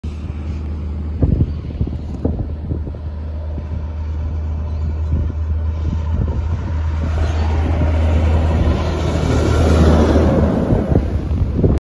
Thurwood refrigerated transport truck
Thurwood refrigerated transport truck heading south bound past Tweed Heads NSW a little while ago